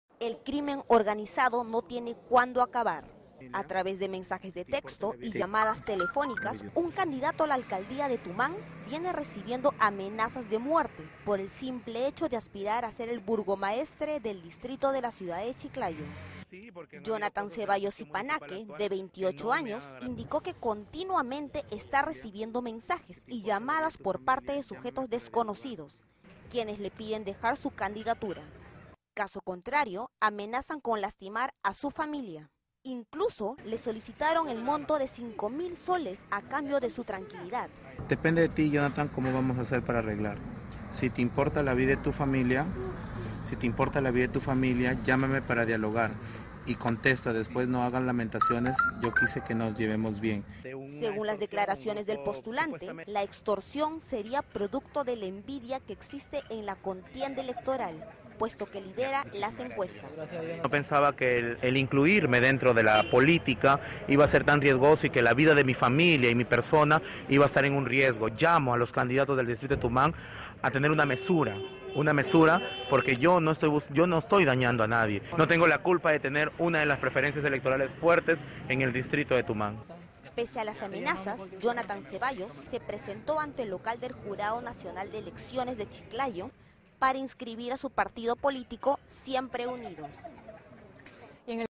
Lambayeque: postulante a la alcadía de Tumán denuncia extorsión
Asimismo, el candidato al sillón municipal de este distrito chiclayano, precisó que los amedentramientos no solo se dan contra él, sino también contra sus familiares.